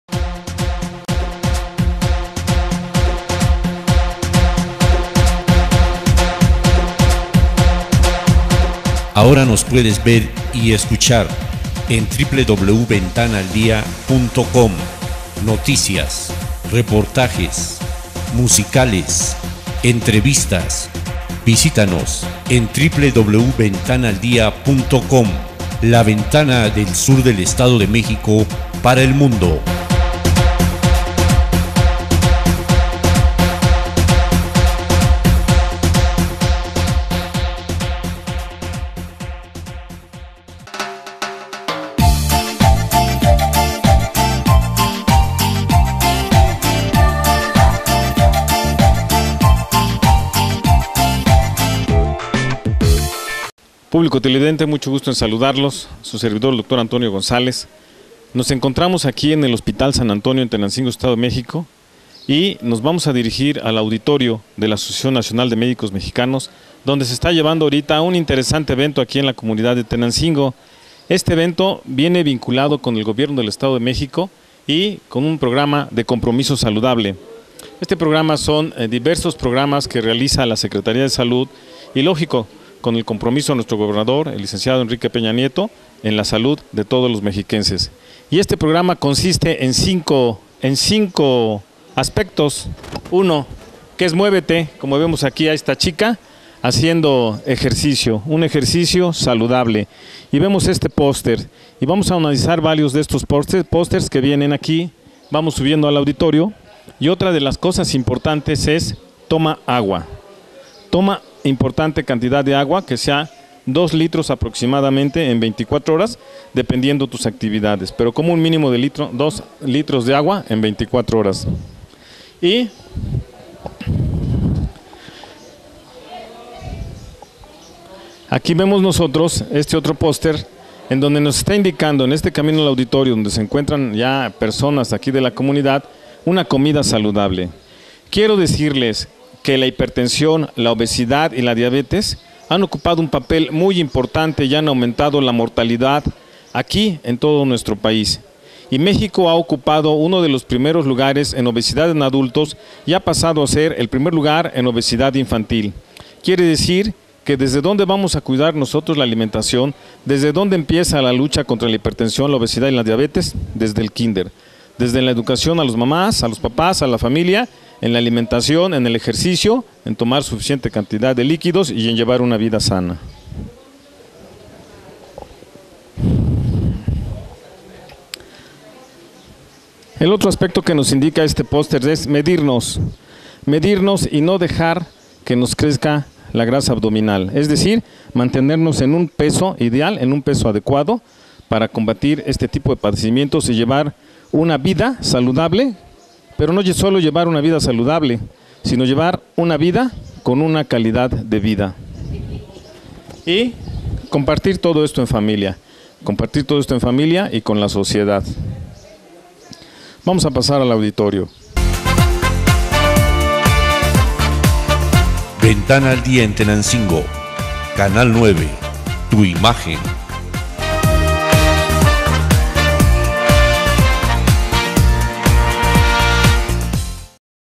TENANCINGO DR ANTONIO GONZALEZ M. COMPROMISO SALUDABLE 2011 01.
COMPROMISO SALUDABLE, UN PROYECTO MAS DEL GOBIERNO DEL ESTADO.
EVENTO REALIZADO EN LA CLINICA SAN ANTONIO DE TENANCINGO  EDO. DE MEXICO